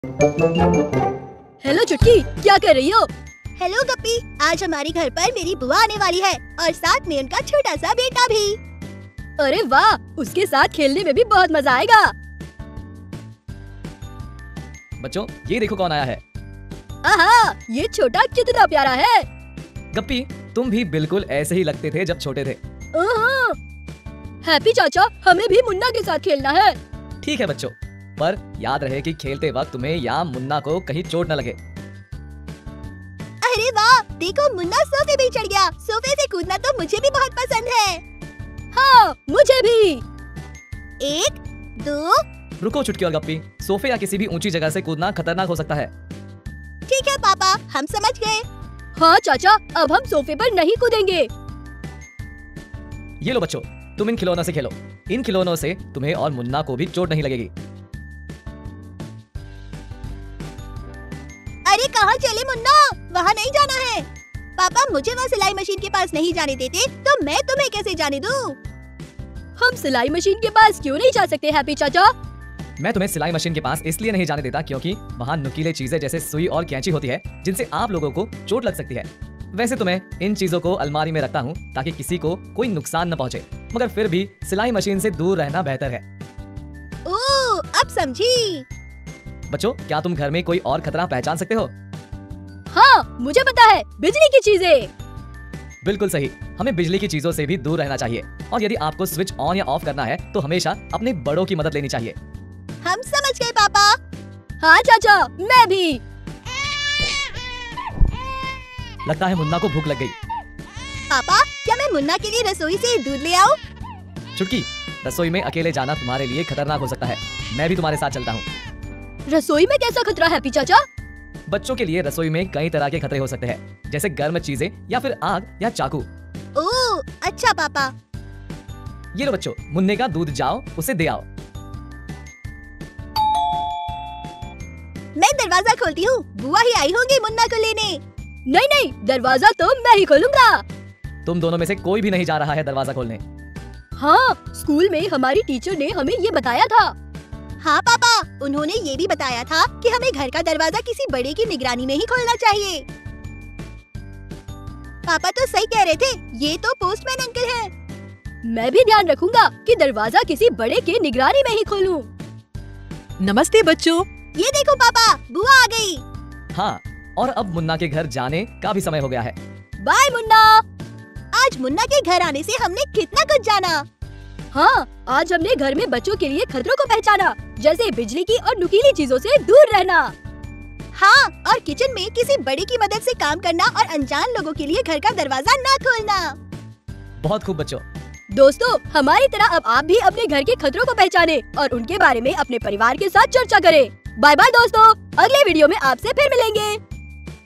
Safety rules at Home 👶🏽🏠 | Safety precautions | Kids' Video | Age 3-8
Timestamps:
0:00 - परिचय
0:03 - बुवा और मुन्ना आ रहे हैं
0:15 - मुन्ना का सभी से परिचय
0:24 - सुरक्षित रूप से खेलना
0:40 - सोफे से कूदने का खतरा
0:52 - सुरक्षित खिलौनों से खेलना
1:01 - सिलाई मशीन के पास प्रतिबंध
1:32 - बिजली के खतरों से बचना
1:59 - रसोई में खतरे
2:21 - दरवाज़ा खोलने में सुरक्षा
2:42 - मुन्ना को अलविदा कहना
3:00 - निष्कर्ष

In this video, we learn about the importance of safety at home.

Join the adorable conversation between two kids as they eagerly await the arrival of their Buwa (aunt) and her little son. The kids find their cousin to be extremely cute and discuss how Gappi used to look the same when he was little. Buwa cautions the children to play with safe toys and not hurt themselves with sharp objects. Later, the kids also identify other potential dangers in the house, such as electrical appliances, and show their understanding by promising to stay away from them. This heartwarming video showcases children's innocence and awareness of their well-being in an engaging and entertaining manner.

इस वीडियो में, हम घर पर सुरक्षा के महत्व के बारे में सीखेंगे।

दो बच्चे अपनी बुवा और उसके छोटे बेटे के आने का बेसब्री से इंतजार कर रहे हैं। बच्चे मुन्ना को बेहद प्यारा पाते हैं और चर्चा करते हैं कि जब गप्पी छोटा था तो कैसा दिखता था। बुवा बच्चों को सावधान करती हैं कि वे सुरक्षित खिलौनों से खेलें और खुद को नुकीली वस्तुओं से चोट न पहुँचाएँ। बाद में, बच्चे घर में बिजली के उपकरणों जैसे अन्य संभावित खतरों की भी पहचान करते हैं और उनसे दूर रहने का वादा करके अपनी समझ दिखाते हैं।